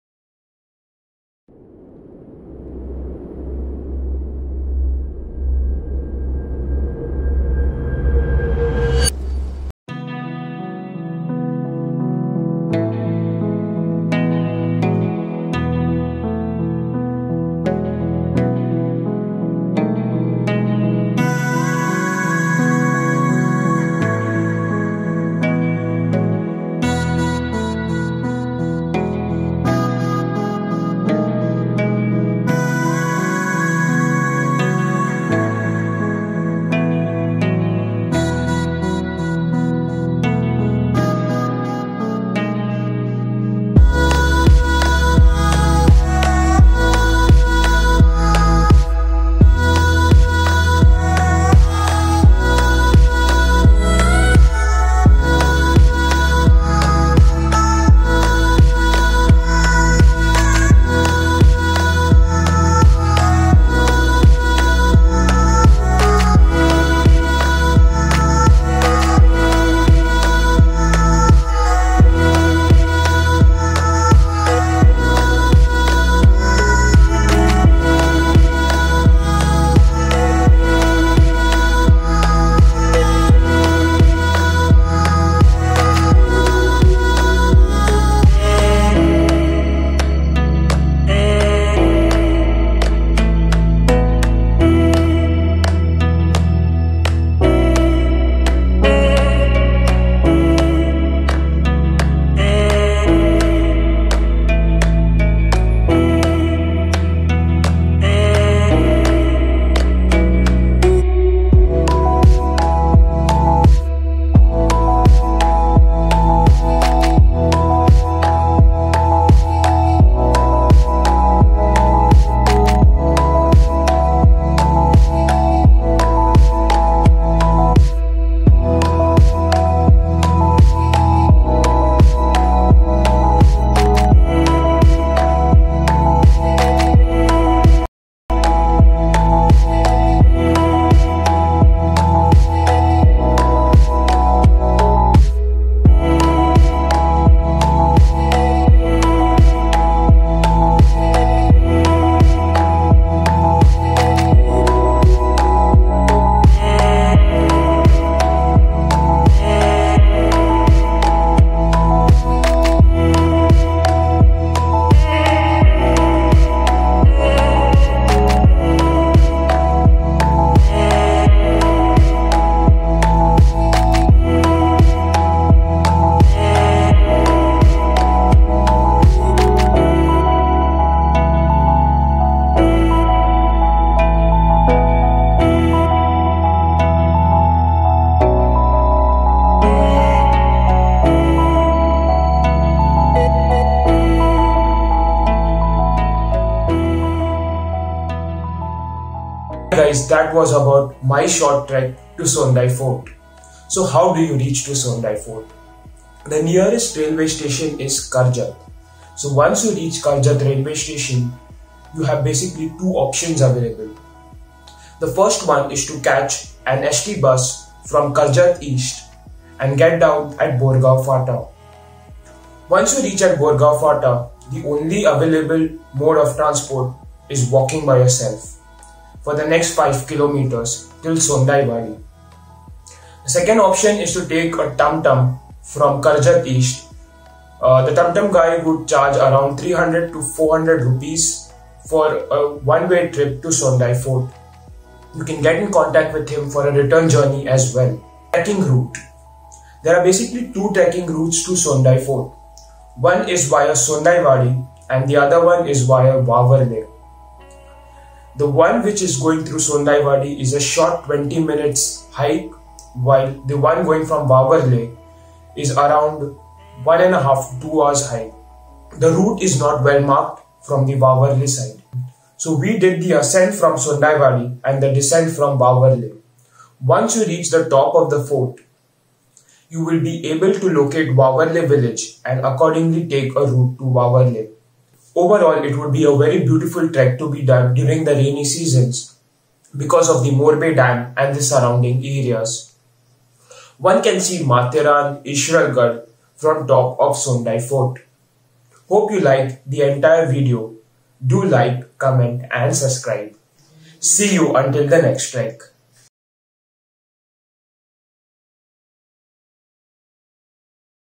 Sondai Fort Trek | Sondaigad | Karjat | Maharashtra Forts| India | An easy hike
Sondai is a small fort near Karjat. There are two routes to reach, the first one being via Sondaiwadi and the next one via Wavarle.

It is an easy 20 min hike from Sondaiwadi and a 2-hour hike from Wavarle.

Route:
Sondaiwadi - Sondai Fort - Wavarle

Video Shot on GoPro Hero 7 Black , one plus 6t & LG cam 360
Edited on Adobe Premiere Pro cc and Adobe After Effects cc.